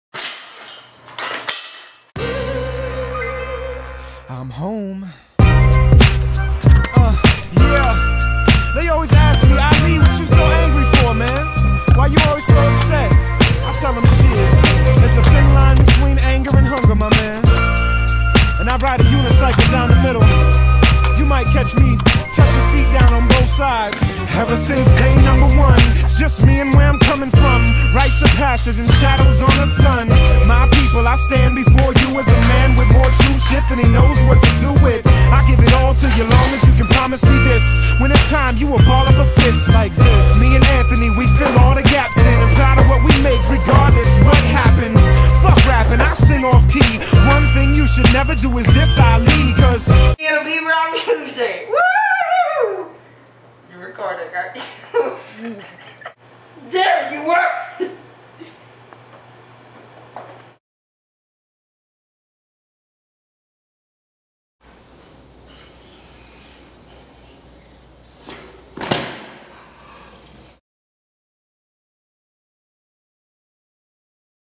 leifs scooter edit
me scootering at my house and a driveway